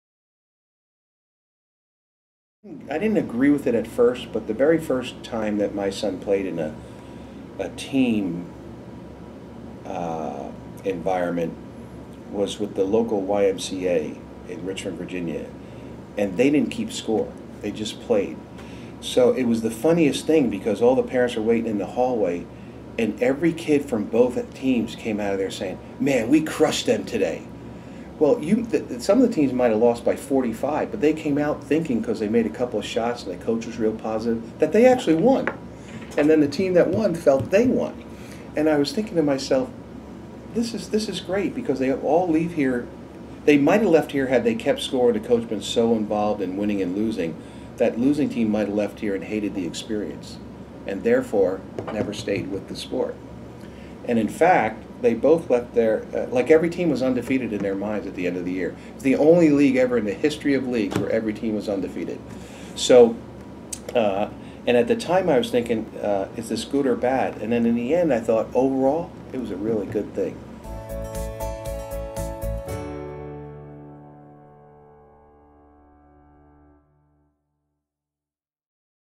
No Score? No Problem
Kevin Eastman, Vice President of Basketball Operations for the LA Clippers, gives his perspective on not scoring a game and the effects of not scoring on the athletes.